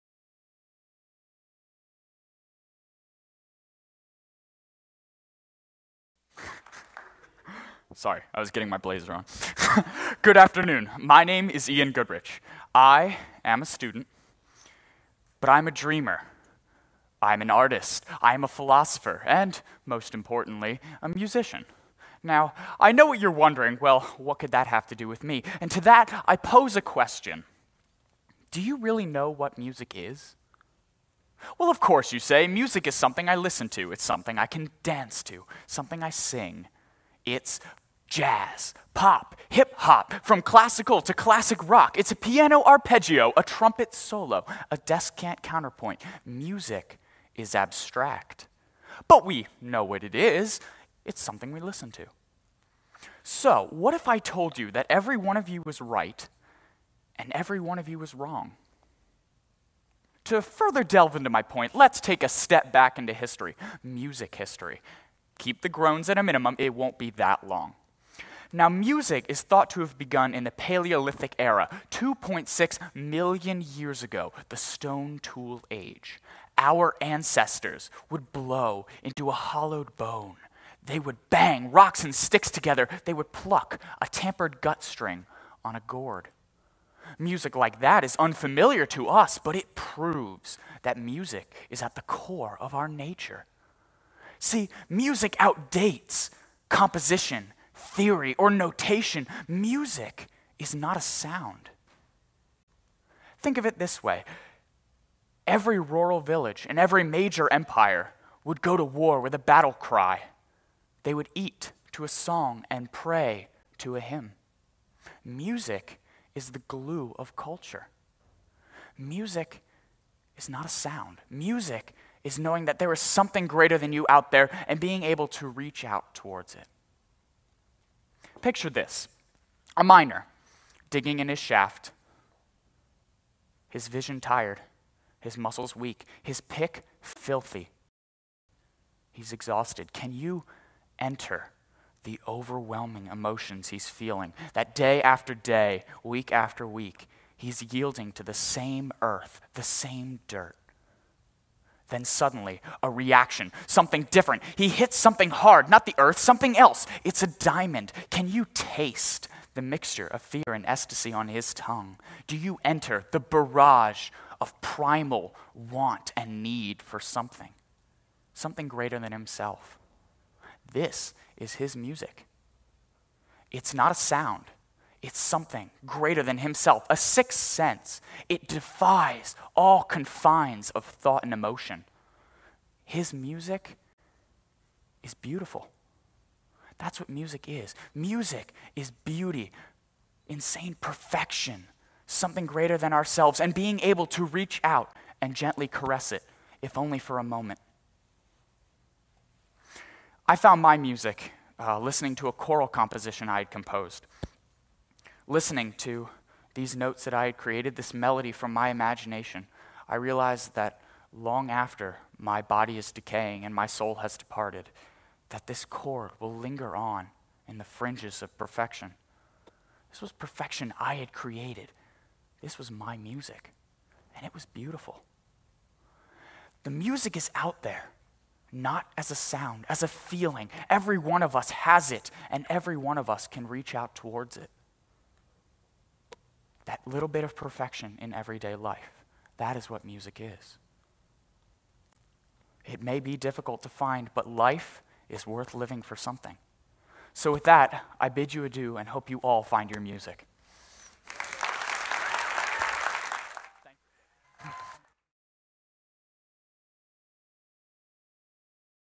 TEDxYouth@CATPickering - Ian Goodrich - Music is the changing word
Ian Goodrich, a senior at Downingtown East High School and student of the Teacher Academy at CAT Pickering, gives a passionate talk about music and his interpretation of it as "a little bit of perfection." Ian has been an active musician his whole life and intends to pursue music in college and as a career.